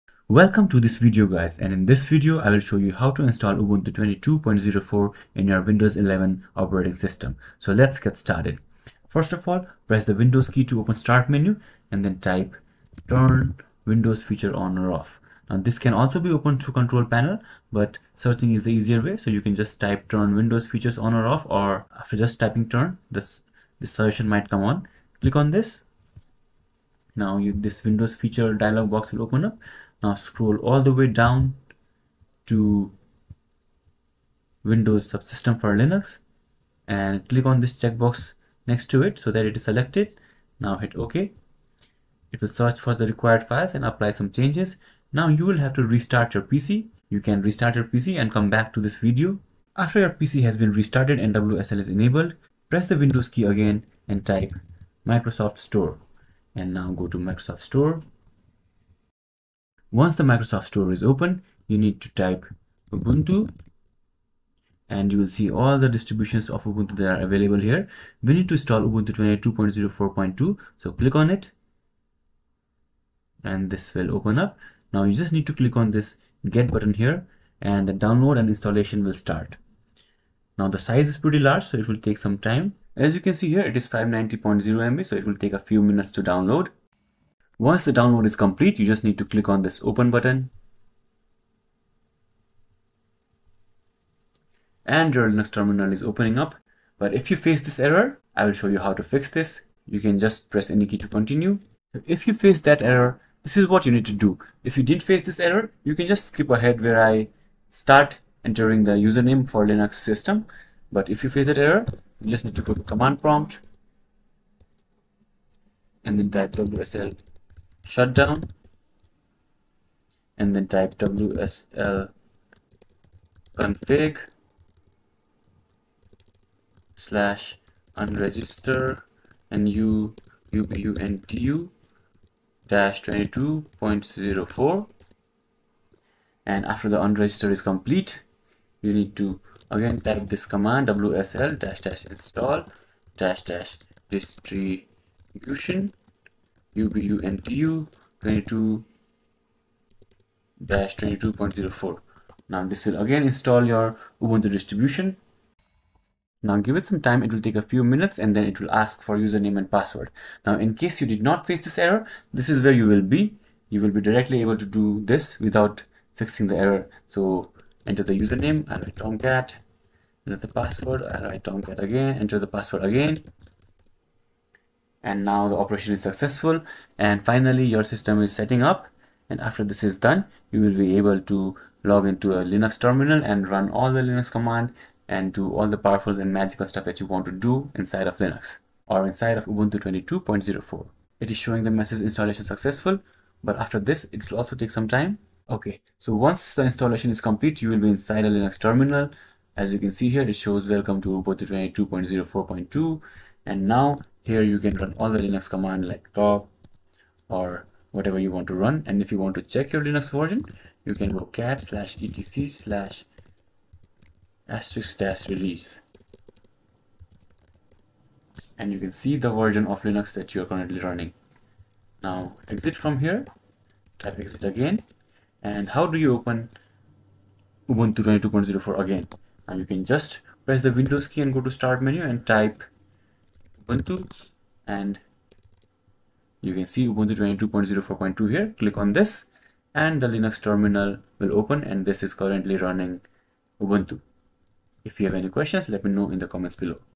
How to Install Ubuntu 22.04 in Windows 11 (Official Method)
In this tutorial video, I'll show you step-by-step on how to install Ubuntu 22.04 on Windows 11. I'll cover the process of enabling WSL (Windows Subsystem for Linux) and demonstrate how to download and install Ubuntu 22.04 from the Microsoft Store. Additionally, I'll also address a common error that users often encounter during the installation process.

By following these instructions, you'll unlock the potential of WSL, a powerful feature that enables you to run a full Linux distribution on your Windows operating system. I'll show you the necessary steps to activate WSL and demonstrate how it simplifies the installation process of Ubuntu 22.04.